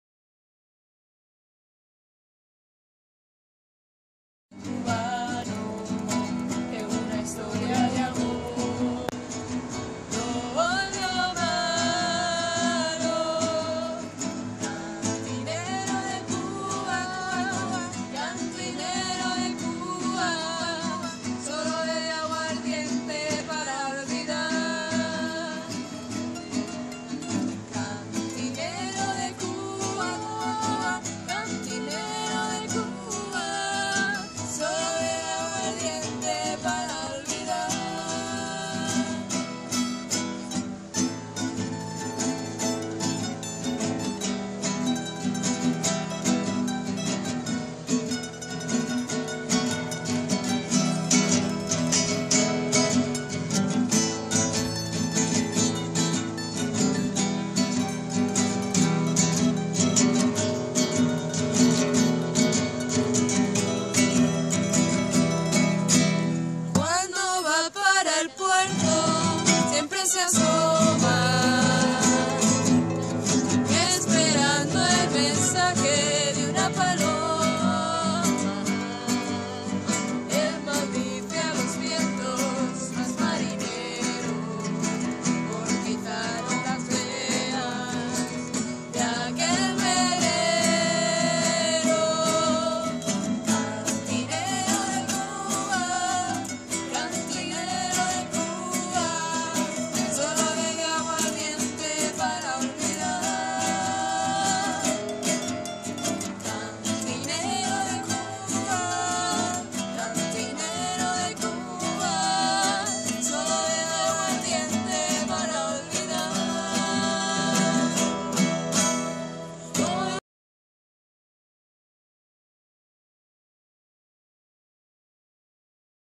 Tres estudiantes universitarias cantando a los pies de la Alhambra
En las tardes frías del otoño, tres jóvenes universitarias desgranan sus cantos en el Paseo de los Tristes, a los pies de la Alhambra,  Granada, España